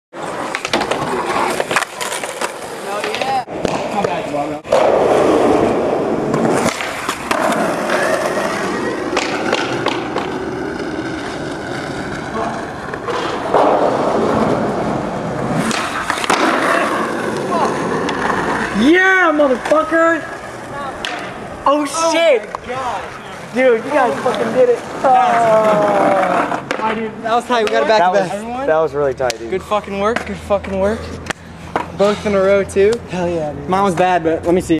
Back to Back
Little trick at westover and some stuff at Staples.